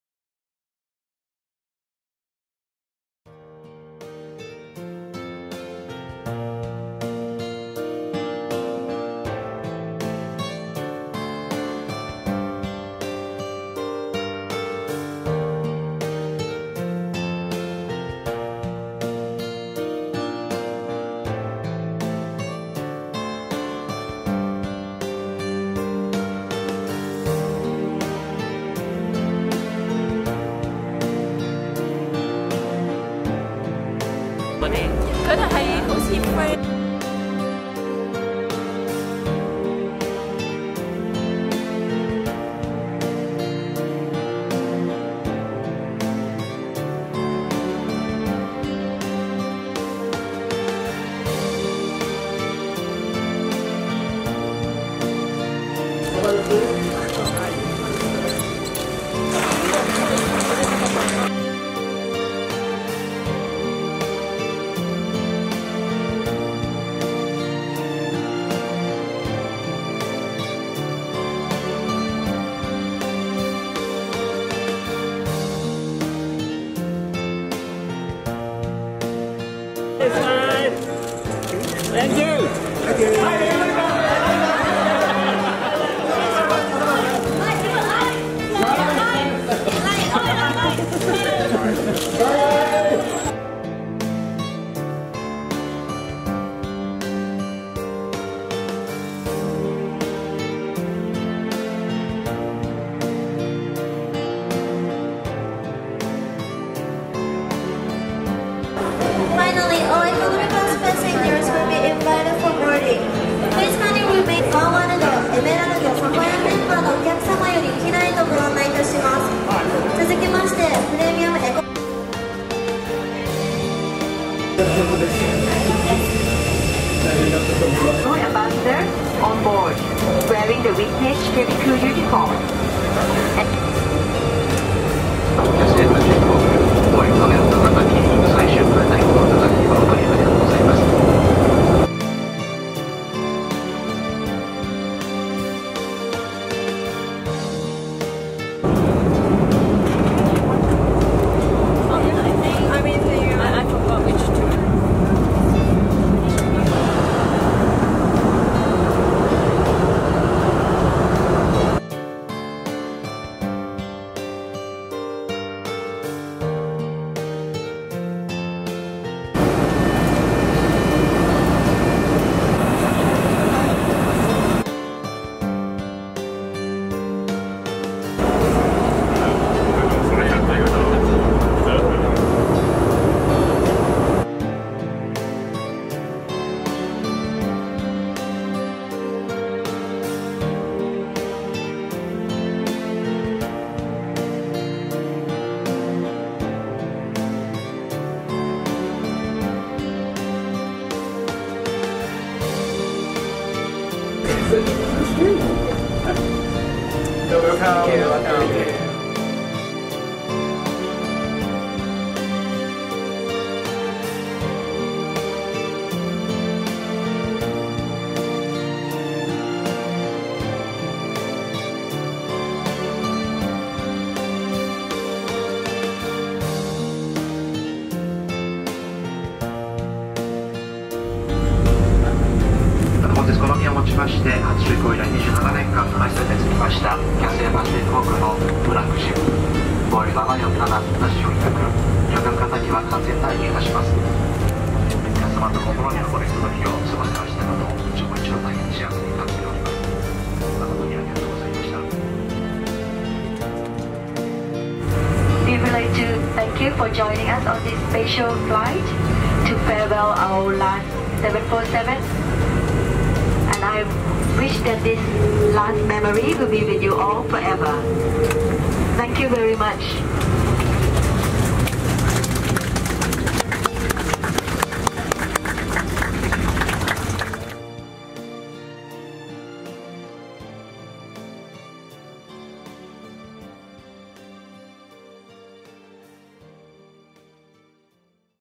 【CATHAY PACIFIC B747 Farewell Flight】キャセイパシフィック航空 B747ラストフライト HND-HKG
Farewell to Our Queen of the Skies.
CATHAY PACIFIC Airways B747-400 Last Flight！
Tokyo Haneda to Hong Kong.

キャセイパシフィック航空の様々な粋な演出、スタッフの皆様には心からお礼申し上げます。

ジャンボの退役は残念ですが、心に残る本当に良いラストフライトでした。
今回は羽田でのチェックインから、香港到着までを動画でまとめました。

少しでもラストフライトの雰囲気が伝わればと思います。

最後にオマケのアナウンスもつけておきましたで、是非最後まで御覧ください。

【撮影で使用した機材】
■Canon EOS 80D

■EF-S18-135㎜ F3.5-5.6 IS USM